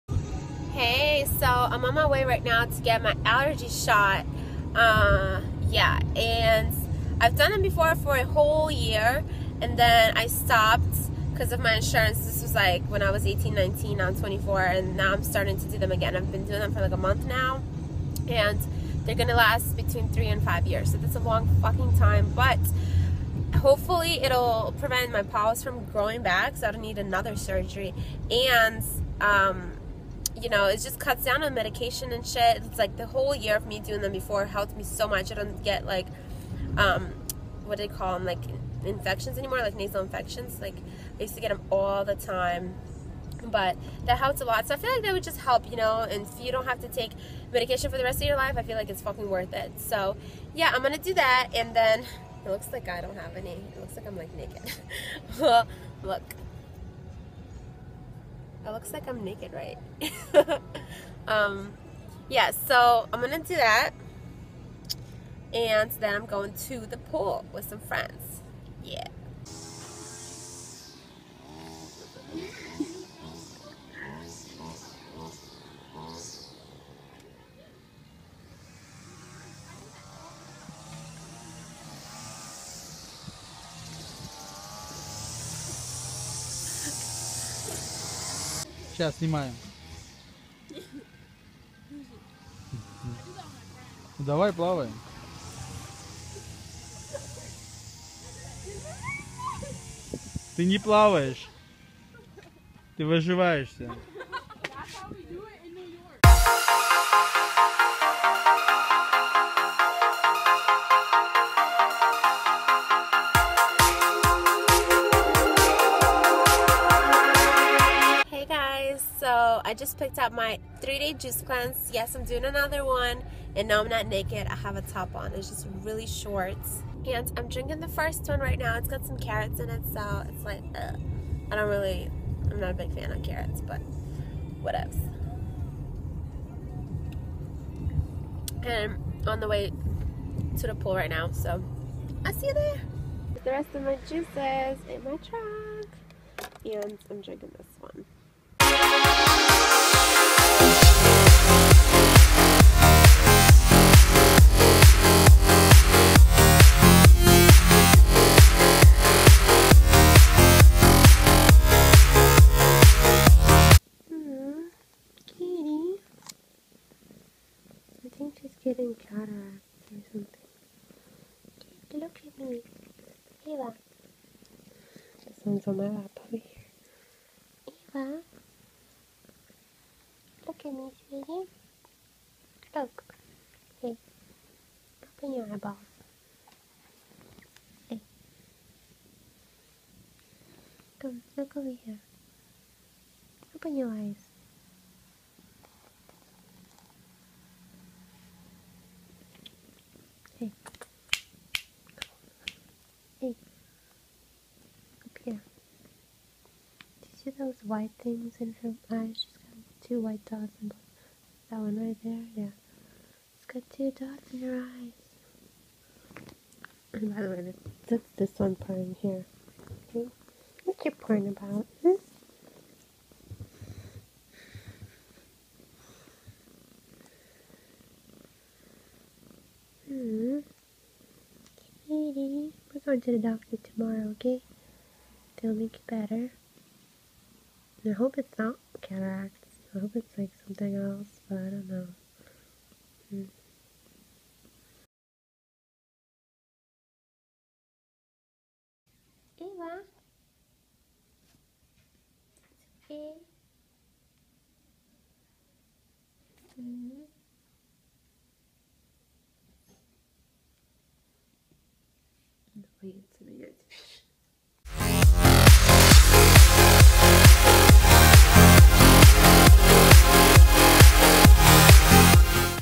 Swimming Pool, Juice Cleanse, Taking My Cat To The Vet, 4th Of July & Russian Parties!
♡POTTERMORE: snitchsky10
♡KEEK: KristinaUA